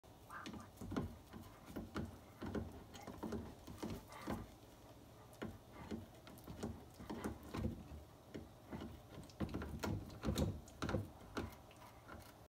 Huskies Are Why We Can’t Have Nice Things
Minnow busy destroying the bar stool.